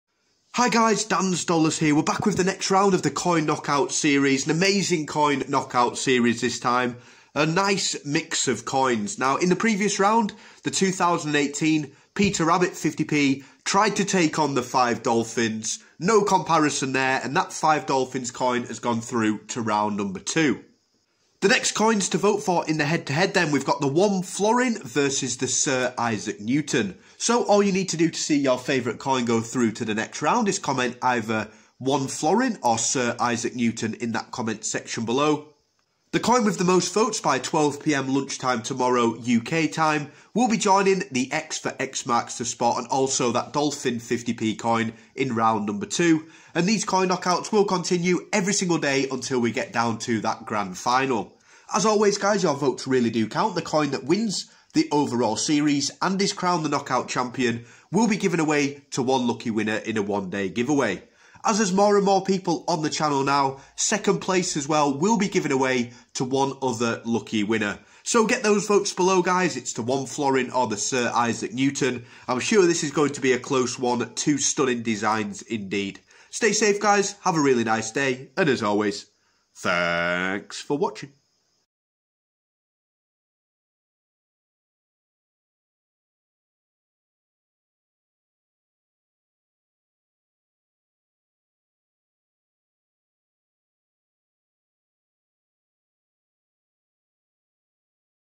20 COINS BUT ONLY 1 CHAMPION #3 - FLORIN vs SIR ISAAC NEWTON 50p coin Knockout!
20 COINS BUT ONLY 1 CHAMPION #3 - FLORIN vs SIR ISAAC NEWTON 50p coin Knockout!! Here we go lady and gentlemen with a new coin knockout series including a jemima puddleduck 50p coin, Isle of man 50p and £2 coins and even some World coins as well. Vote either ONE FLORIN vs SIR ISAAC NEWTON 50p coin Knockout!